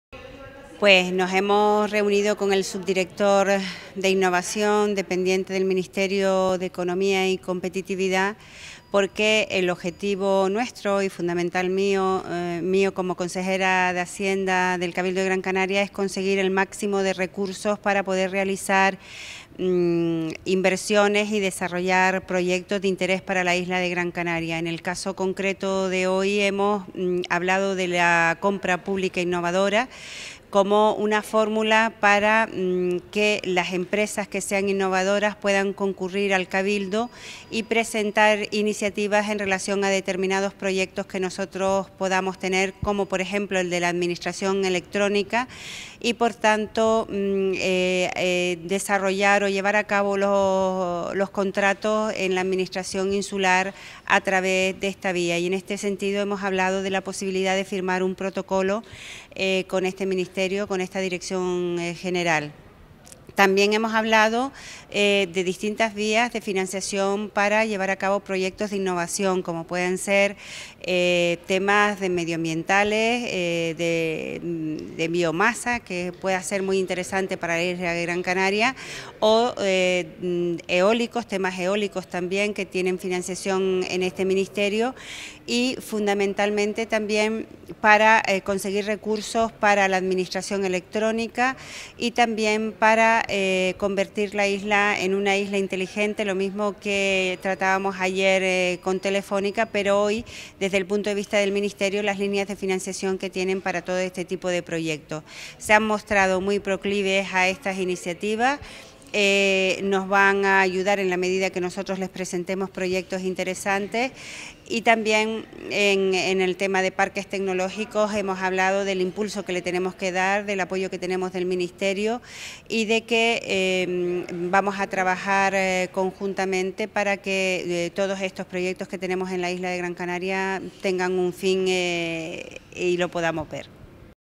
Rosa Rodríguez quiere fomentar la innovación empresarial en Gran Canaria
La vicepresidenta II y consejera de Gobierno de Presidencia, Economía y Hacienda del Cabildo de Gran Canaria, Rosa Rodríguez Díaz, declaró hoy en Madrid que la compra pública innovadora es un instrumento muy interesante para fomentar la innovación empresarial en Gran Canaria, "y estamos barajando firmar un protocolo de desarrollo al respecto con la Subdirección General de Fomento de la Innovación Empresarial del Ministerio de Economía y Competitividad".

La iniciativa se enmarca en el encuentro de trabajo que mantuvo la vicepresidenta con el subdirector general de Fomento de la Innovación Empresarial del Ministerio de Economía y Competitividad, Luis Cueto Álvarez de Sotomayor, con el objetivo de conseguir el máximo de recursos para realizar inversiones y desarrollar proyectos de interés en Gran Canaria.

Al término de la reunión, Rodríguez explicó que la compra pública innovadora es una actuación administrativa de fomento de la innovación que trata de orientar las compras que realizan las administraciones públicas, no sólo al cumplimento de sus fines, sino al fomento del desarrollo tecnológico de las empresas, incentivándolas a que hagan propuestas más innovadoras en su oferta.

Esta fórmula puede convertirse en un instrumento para potenciar el desarrollo de los mercados innovadores, como nuevos elementos tractores del crecimiento económico, y además puede aplicarse a proyectos que desarrolla en la actualidad el Cabildo de Gran Canaria, como es el de Administración Electrónica.

Sobre esto, Rosa Rodríguez trasmitió al representante del Ministerio la importancia de conocer otras experiencias de éxito y lograr los apoyos para liderar un proyecto de administración electrónica de todas las entidades locales de la isla bajo el paraguas del Cabildo de Gran Canaria. "Alvarez de Sotomayor ha sido muy receptivo a las iniciativas de esta Corporación y se ha mostrado colaborador en la medida en que nosotros le presentemos proyectos interesantes", concluyó.

Sobre administración electrónica, Rosa Rodríguez apuntó que actualmente en Gran Canaria hay una falta de homogeneización en la política de modernización de dichas entidades locales, cada municipio, o carece de ella o tiene una solución diferente de administración electrónica que dificulta la colaboración entre todas, así como la reutilización de conocimientos y recursos, y el ahorro de costes. Nuestro reto es pues, hacer converger las líneas de modernización de los 21 ayuntamientos de Gran Canaria para facilitar la obtención de una administración local de calidad y eficiente bajo una misma plataforma de E-Administración.

En este empeño, el Cabildo de Gran Canaria ha mantenido desde el pasado mes de enero y dirigidas por la vicepresidenta, varias reuniones con el Ministerio de Economía y Competitividad, para estudiar las alternativas de financiación a la investigación e innovación existentes por parte del Estado, a las que Gran Canaria puede acogerse para materializar este y otros proyectos.

Por eso en la reunión de hoy se insistió también en las distintas vías de financiación para acometer proyectos de innovación en temas como el medioambiente, bio-masa o energía eólica, en la medida en que pueden ser interesantes para Gran Canaria, así como en impulsar el desarrollo de los parques tecnológicos de la isla, para lo que el Ministerio también ha expresado su total apoyo y la idea de trabajar conjuntamente para que todo este esfuerzo llegue a buen puerto